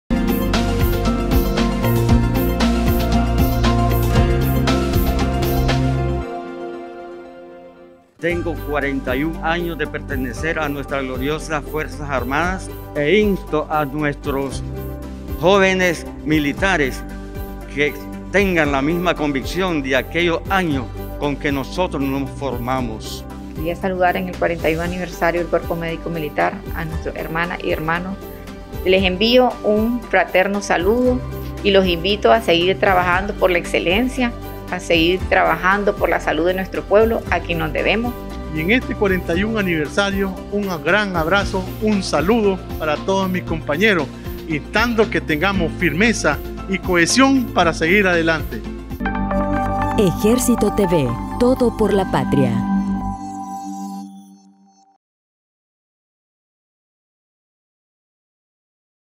Saludos al Cuerpo Médico Militar en conmemoración de su 41 aniversario de constitución (Pt. 4)
En el marco del 41 aniversario de constitución del Cuerpo Médico Militar, miembros, doctores, cadetes, enfermeras y personal auxiliar envían sus saludos.